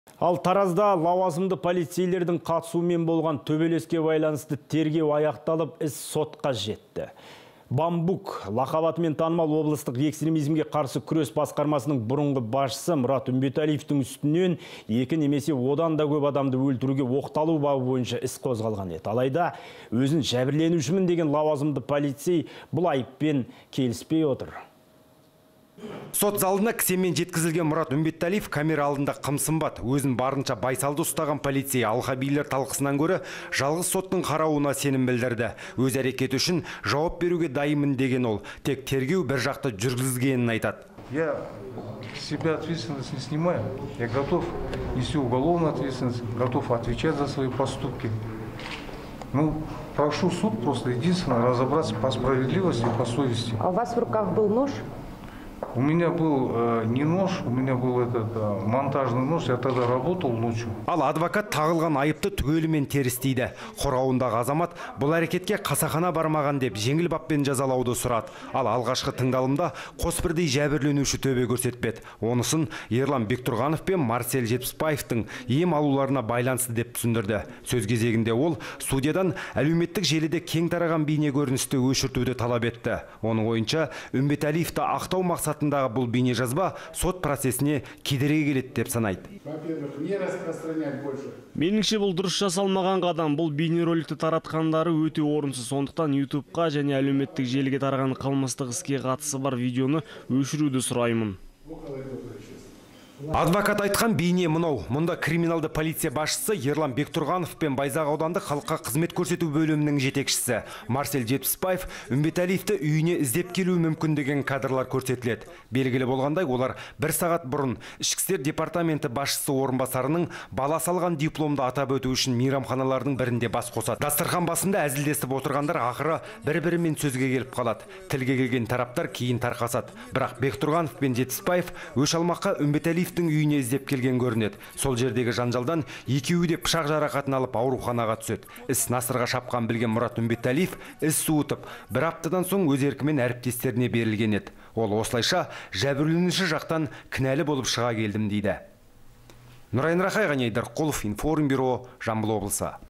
«Бамбуктың» бабы
Таразда лауазымды полициейлердің қатысуымен болған төбелеске байланысты тергеу аяқталып, іс сотқа жетті. «БАМБУК» лақап атымен танымал облыстық экстремизмге қарсы күрес басқармасының бұрынғы басшысы Мұрат Үмбеталиевтің үстінен «екі немесе одан да көп адамды өлтіруге» оқталу бабы бойынша іс қозғалған еді. Алайда өзін «жәбірленушімін» деген лауазымды полицей бұл айыппен келіспей отыр.